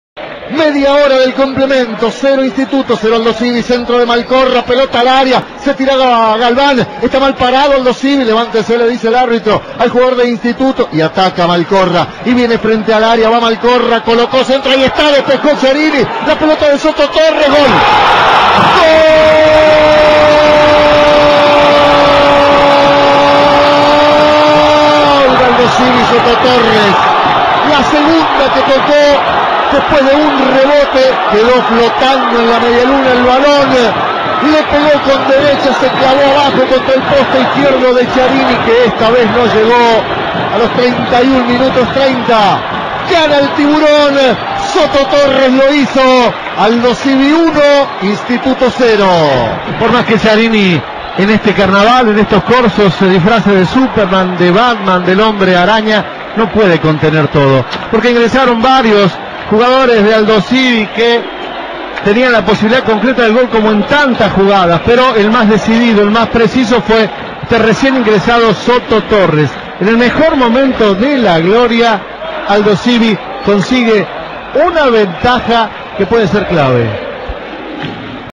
Aldosivi 1 Instituto 0 (Relato Hugo Lombardi) Torneo Nacional B 2014 (2/3/2014)
El gol del partido con relato de Hugo Lombardi Cadena 3 La Cadena del Gol